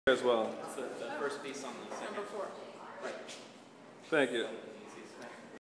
Lawrence Schiller at 'Block Party 2012' Hosted By The Hor...
Lawrence Schiller at 'Block Party 2012' Hosted By The Horticultural Society of New York at Central Park Arsenal on June 18, 2012 in New York, New York